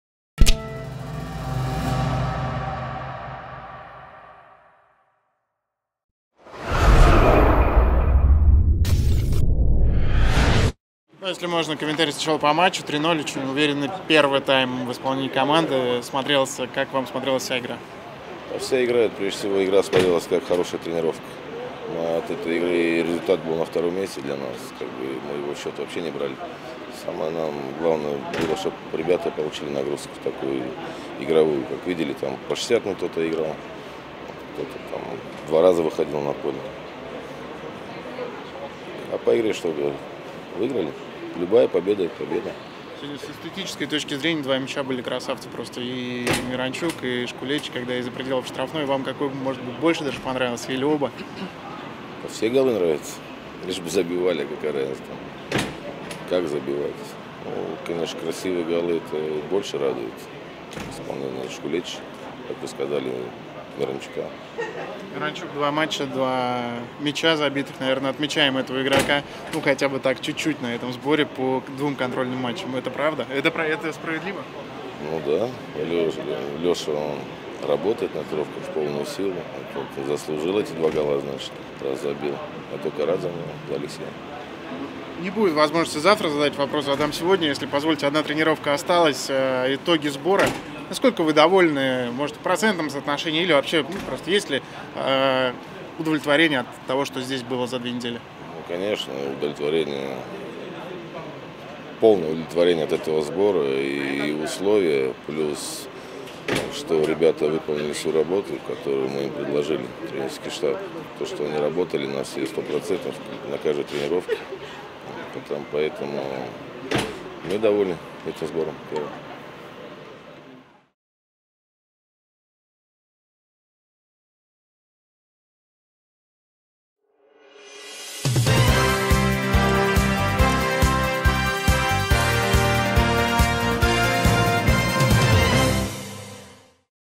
Черевченко: Мы довольны сбором в ОАЭ
Главный тренер «Локомотива» Игорь Черевченко резюмировал итоги сбора в ОАЭ и матча с «Бунедкором».